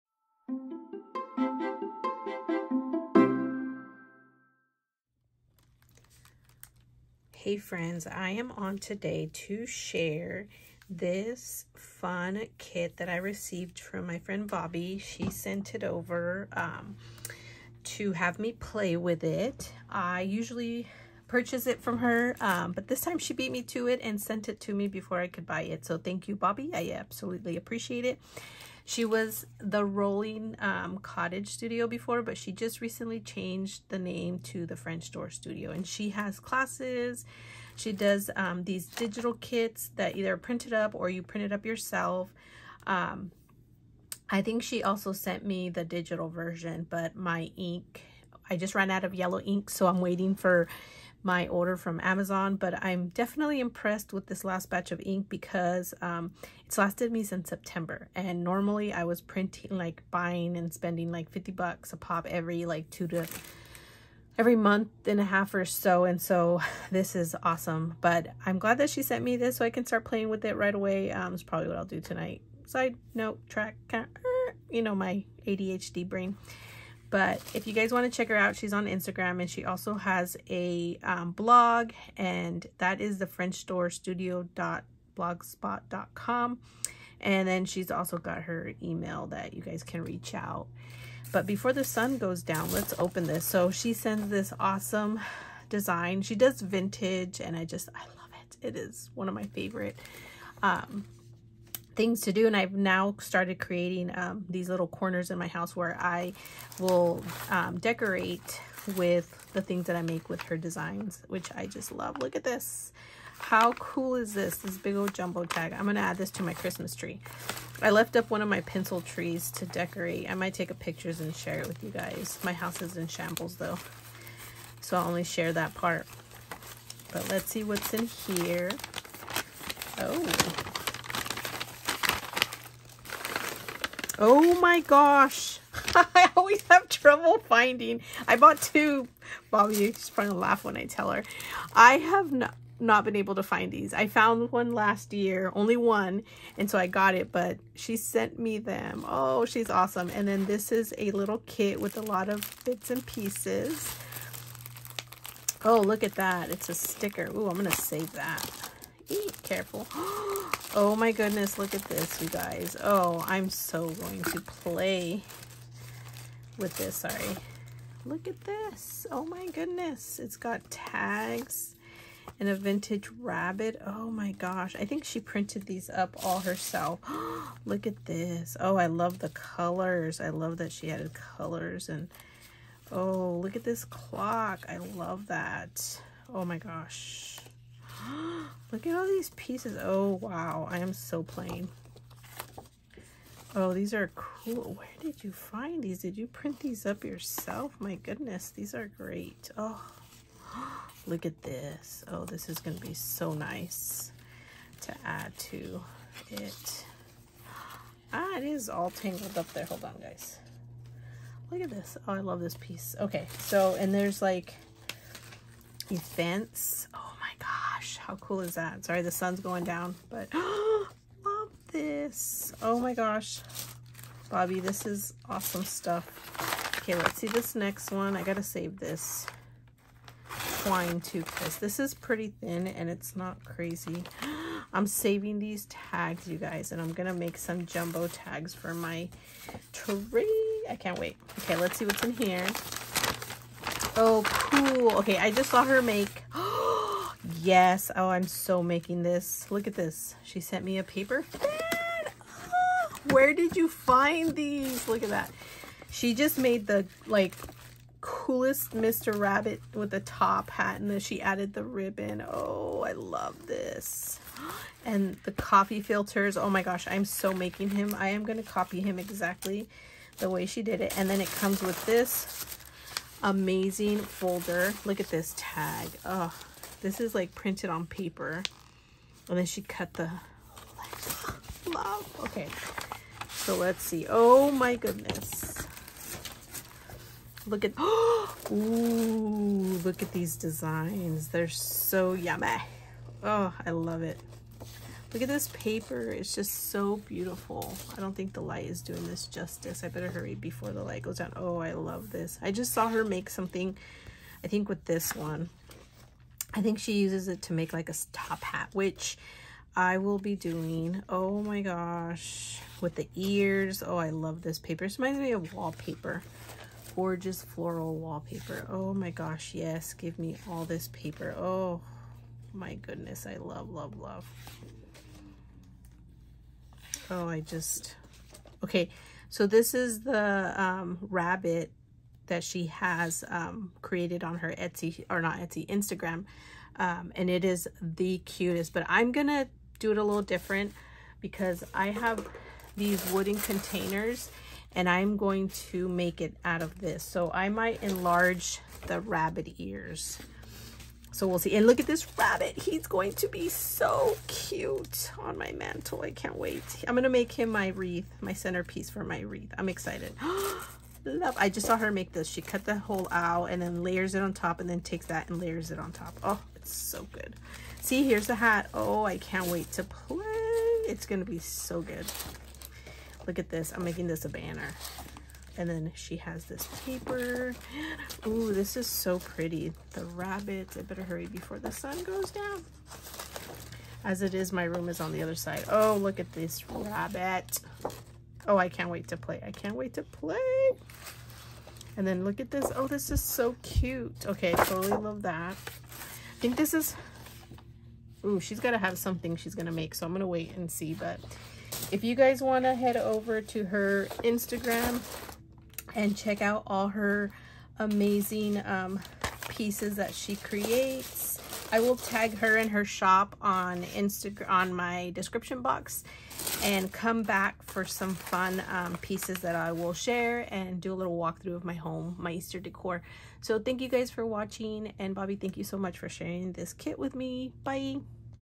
Rabbit Rabbit Spring Collection by The French Door Studio | Digital Art Kit | Vintage Easter 🐣
Hi!
Thanks for stopping by and taking a look at my haul review of this awesome Kit I received from Bobbie from the French Door Studio.

Thank you for stopping by and I hope you are inspired!